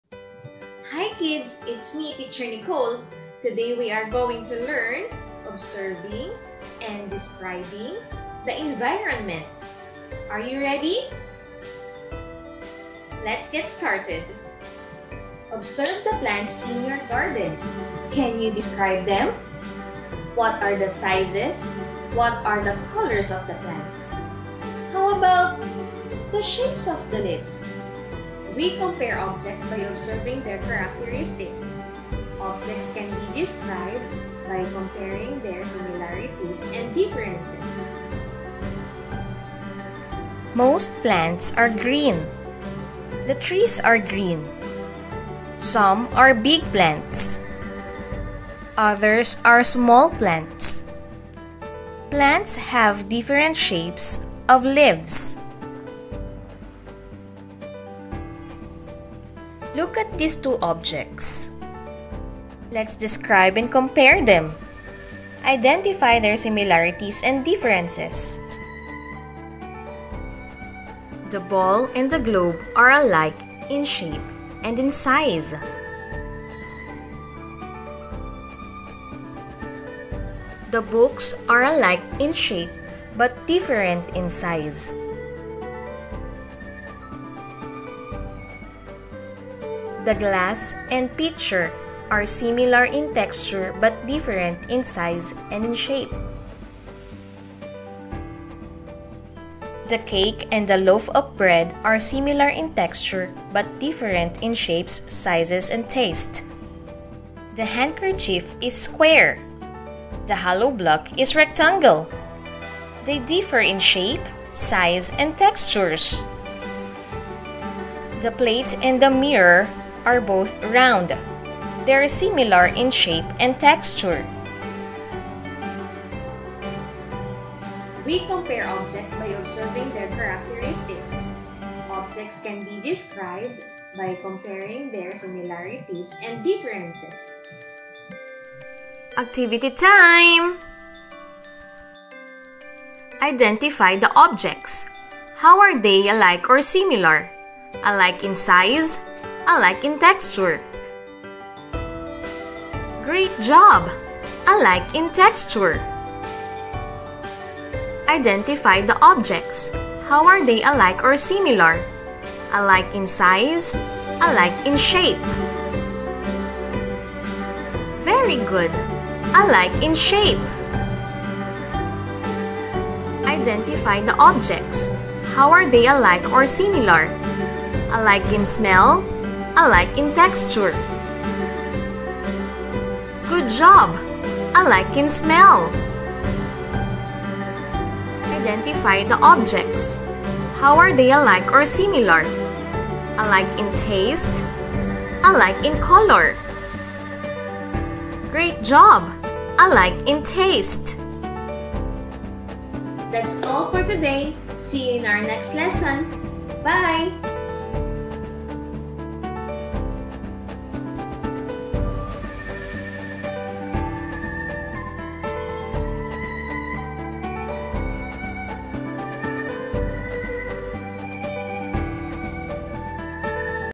Observing and Describing the Environment for Kindergarten
Learning in Science  involves different processes. Observation is the most important skills in science.Observation is learning things around through the senses. We observe to identify and describe. We observe to compare and classify.